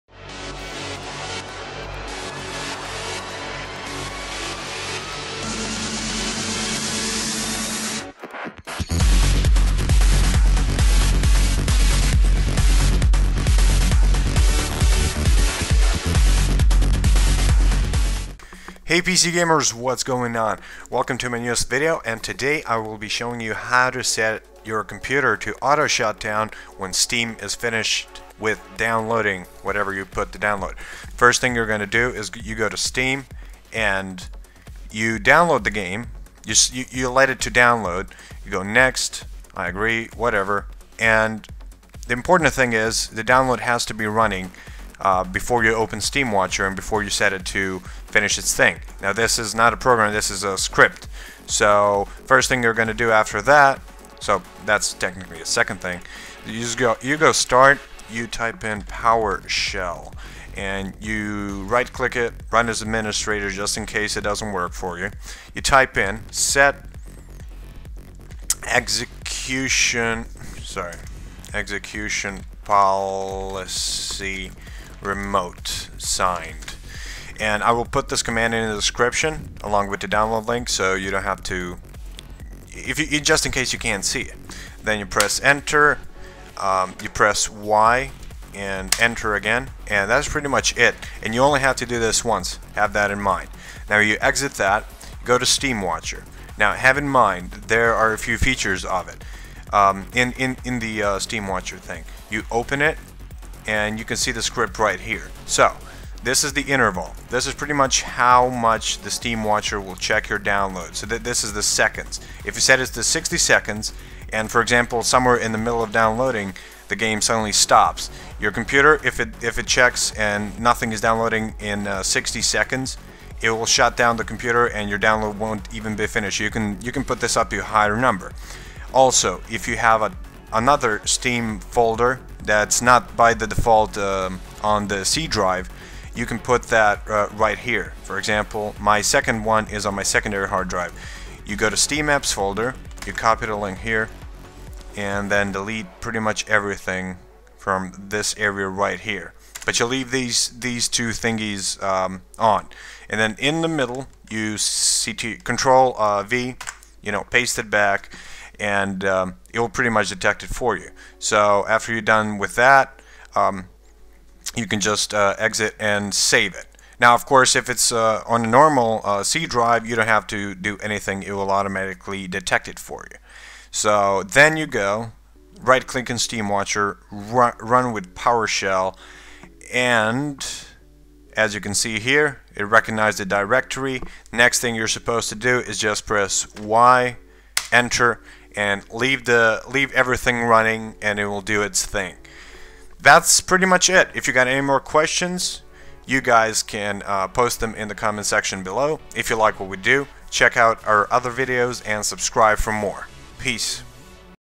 How to Auto Shut Down PC after STEAM Stops Downloading - Windows 7
A simple program that will help you in case you want your PC to shutdown after steam is done with downloading, but you don't wanna be there when it happens :) You can download it here:

Powershell command:
set-executionpolicy remotesigned

External HDD

The process of the script is:
1. Check Registry for steamapps directory location on computer
2. Get user to confirm location is correct
3. Begin Loop
4. Check Size of Steamapps directory
5. Wait 60 seconds
6. Check size of steamapps directory again
7. Did the directory grow during the 60 seconds? If yes, go to step 4. If not, go to step 8.
8 .Computer should shut down